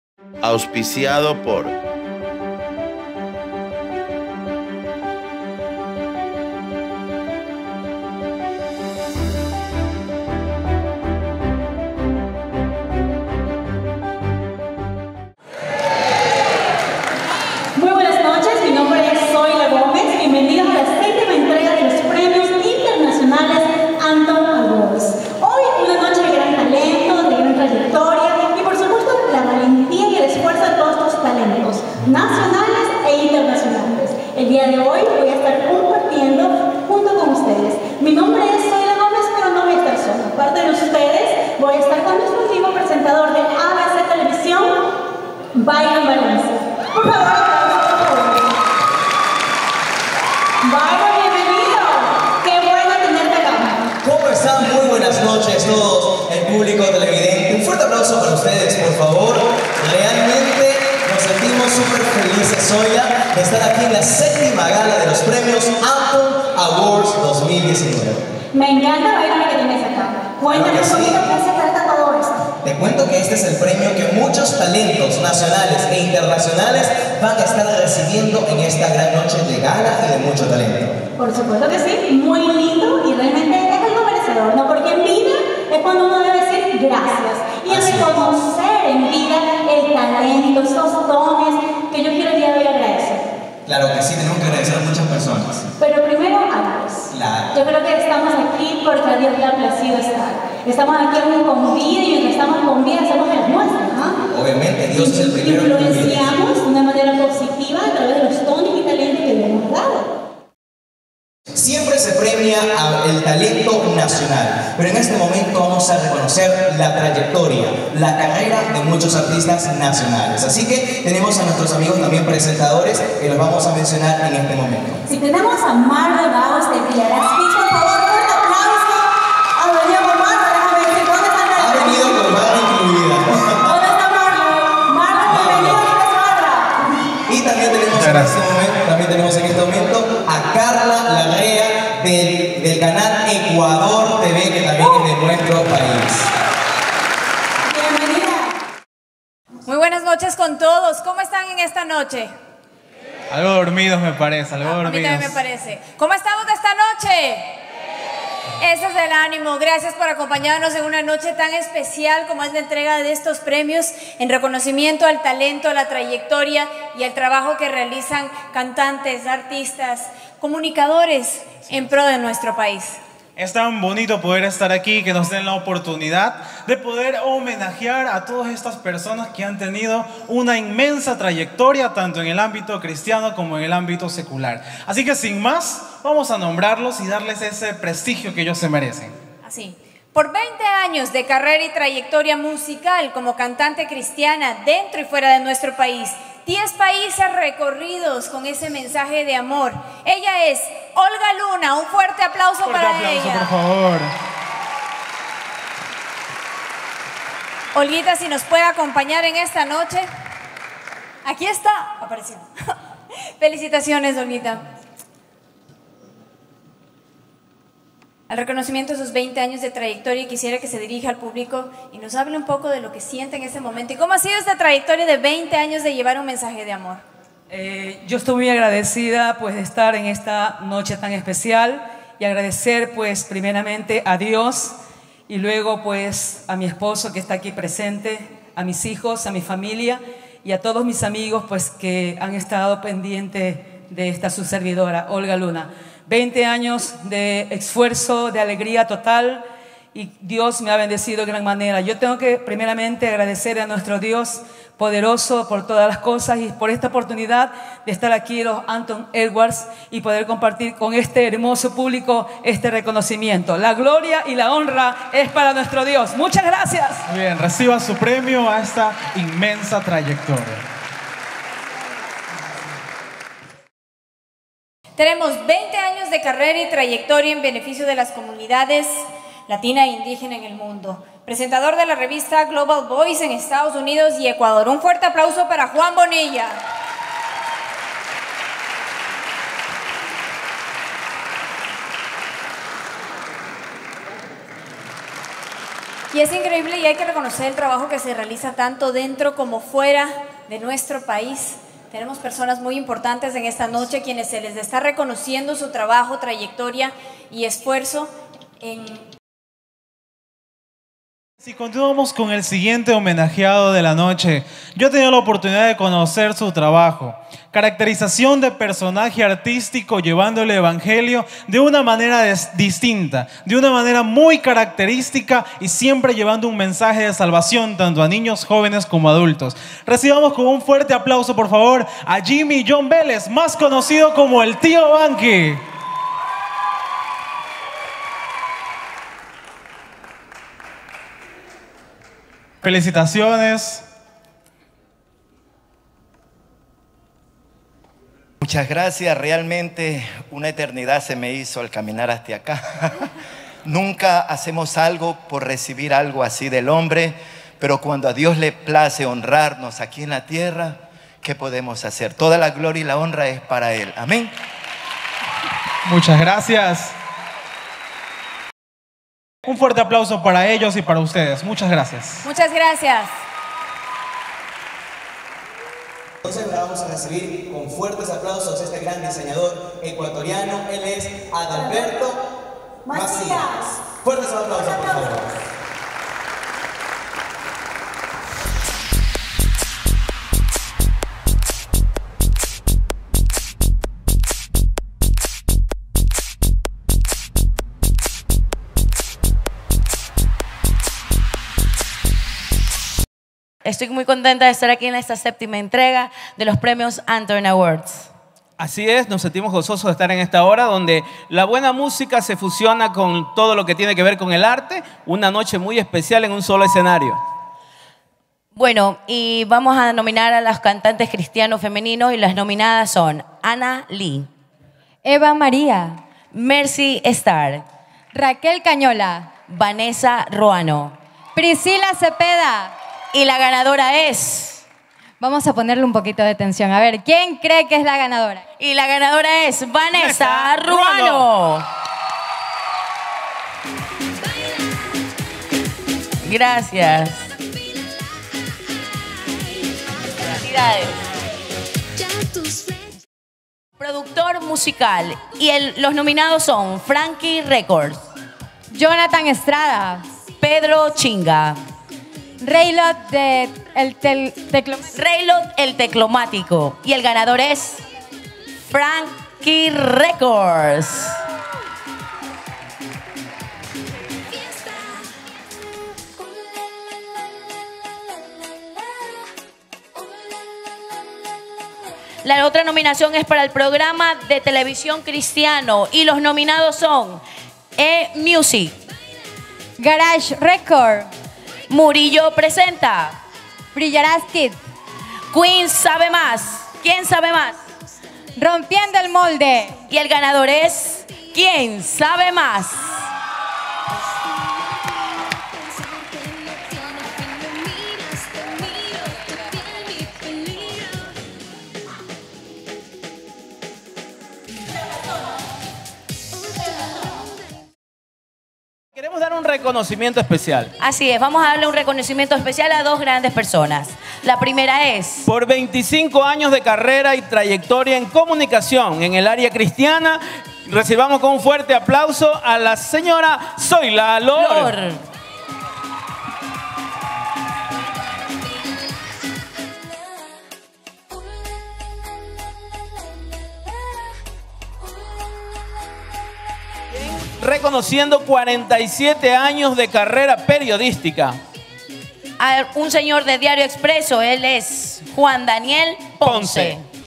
PREMIACION DE LOS ANTON AWARDS ECUADOR 2019 - PARTE 2
PRESIDENTE MICHEL PLANCHART
Guayaquil Ecuador - Teatro Centro Cívico
Domingo 26 de Octubre del 2019